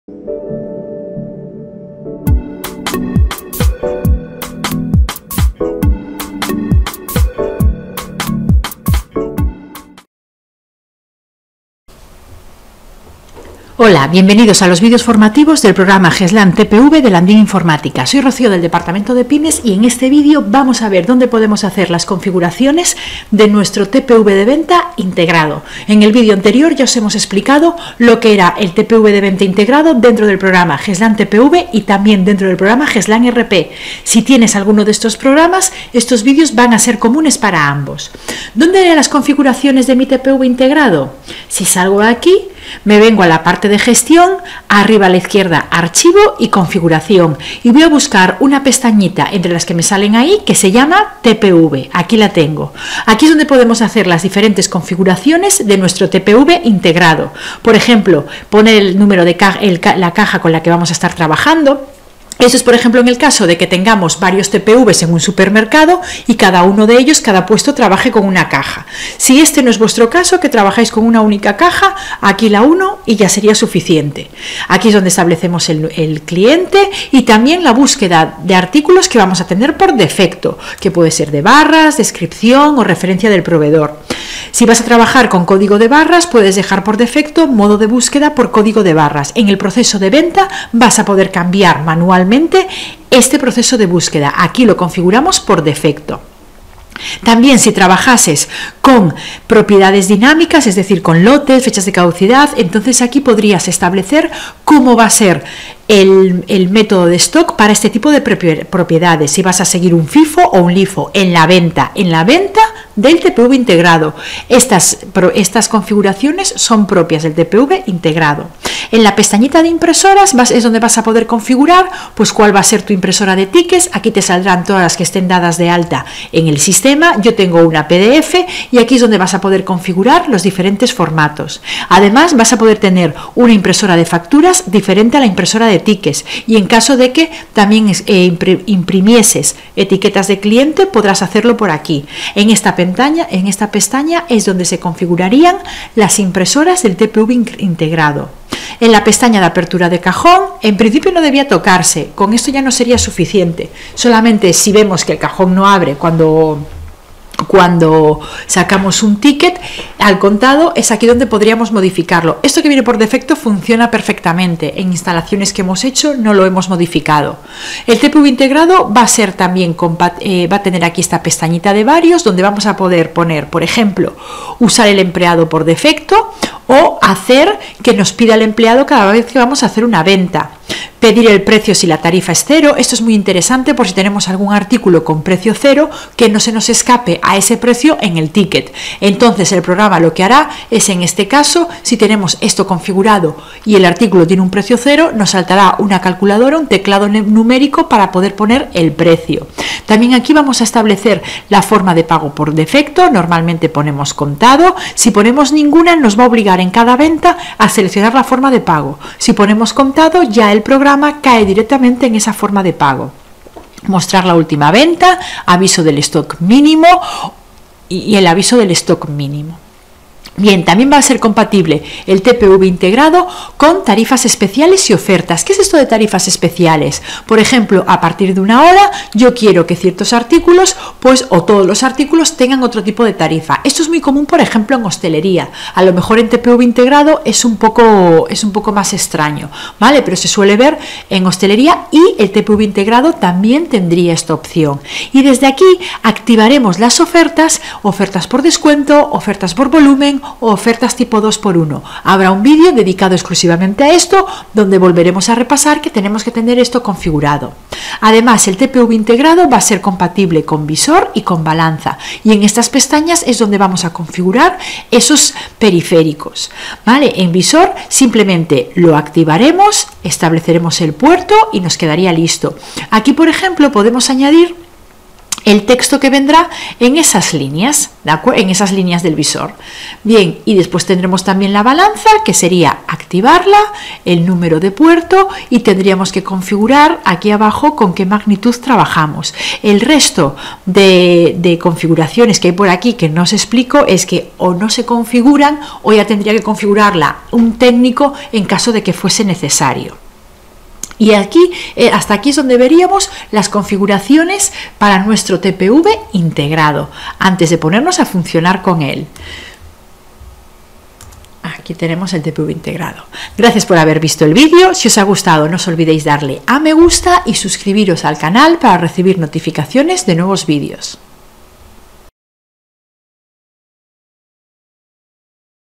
CONFIGURACIONES BÁSICAS TPV INTEGRADO
- Cómo configurar el tpv integrado con las opciones de venta
- Congiguración de impresora